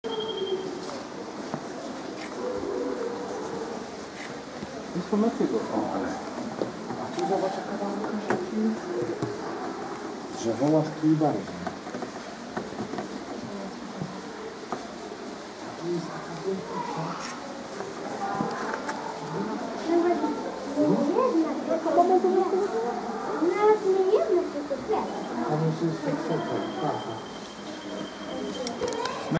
Yellow Banded Poison Dart Frog at Stare Poznan Zoo
Yellow-banded poison dart frog (Dendrobates leucomelas) at Stare (old) Poznan Zoo. Also known as yellow-headed poison dart frog or bumblebee poison frog, it's a poisonous frog from the Dendrobates genus of the Dendrobatidae family. Found in the northern part of continent of South America, most notably in Venezuela